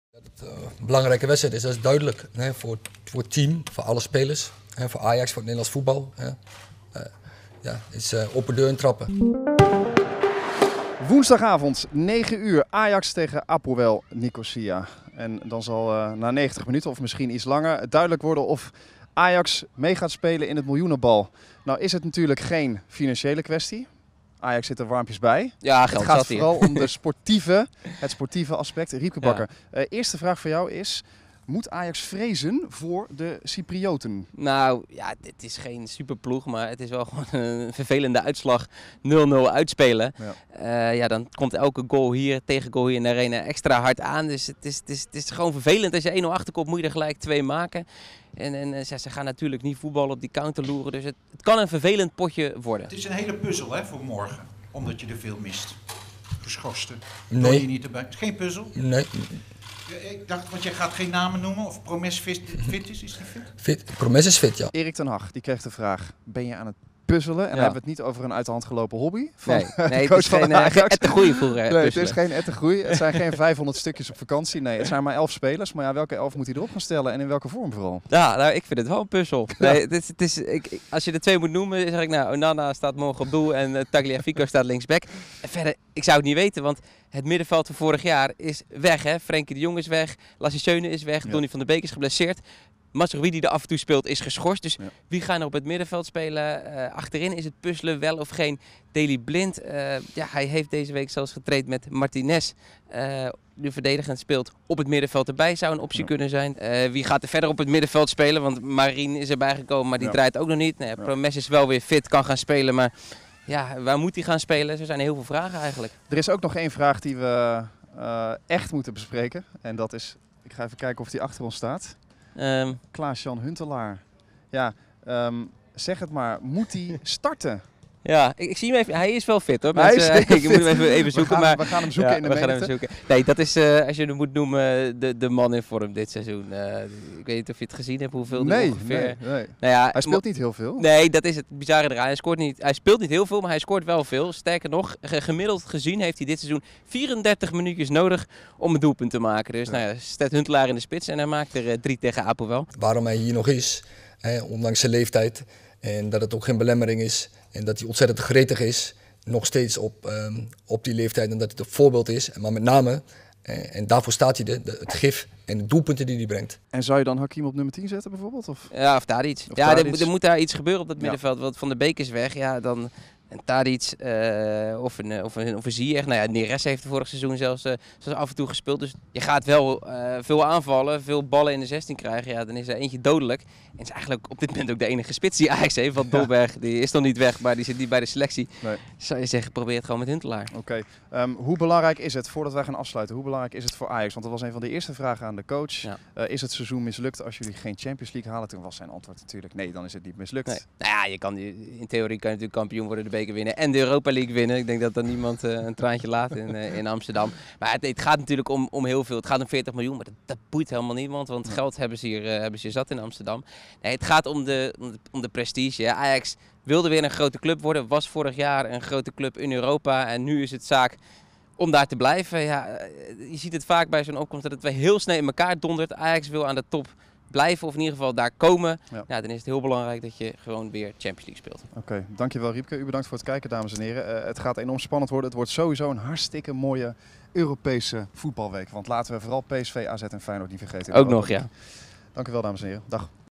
'40 miljoen euro erbij boeit niemand bij Ajax' | Vooruitblik Champions League Ajax - Apoel | NU.nl
Ajax zal woensdagavond in eigen huis moeten winnen van APOEL Nicosia om zich te plaatsen voor de poulefase van de Champions League. Het is voor coach Erik ten Hag hoe dan ook een puzzel, gezien de blessure van Donny van de Beek en de geschorste Noussair Mazraoui.